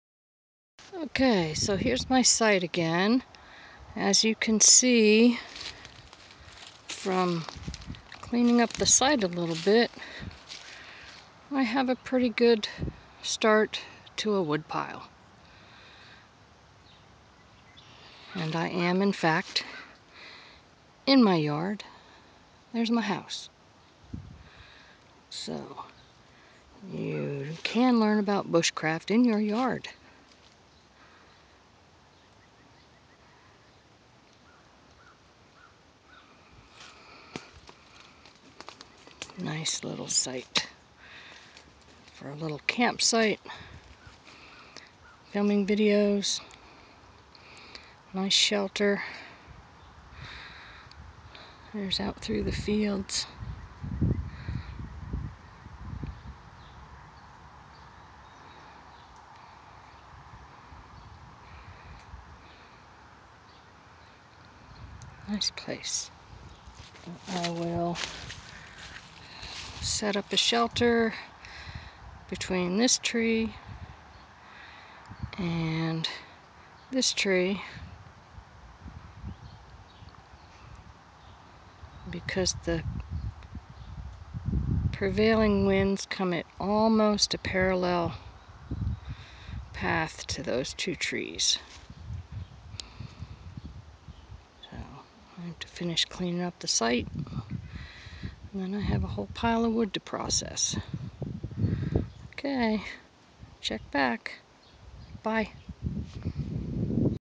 A little video of clearing up the Basic Backyard Bushcraft campsite.